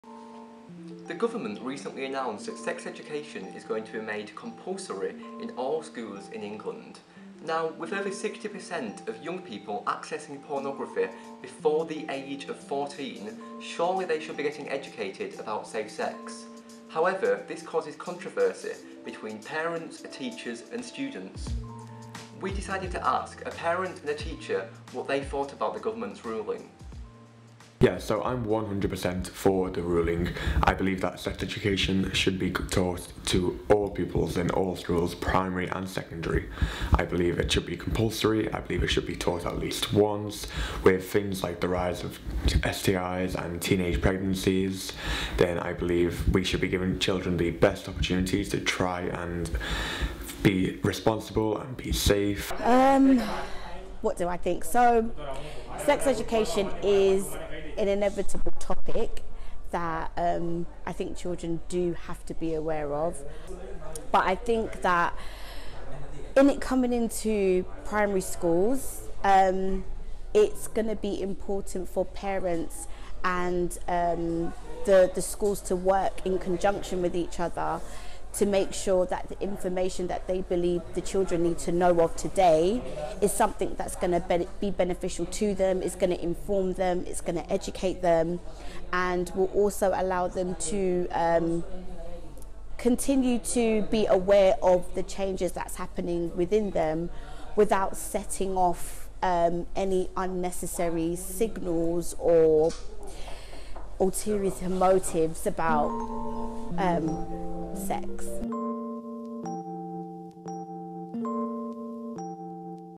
Sex education in schools: What do teachers and parents think?
Education secretary Justine Greening announced that sex education will be made compulsory form the age of four in English schools. Our reporter Lewis Parker spoke to a teacher and a parent to see what they think about the new measure.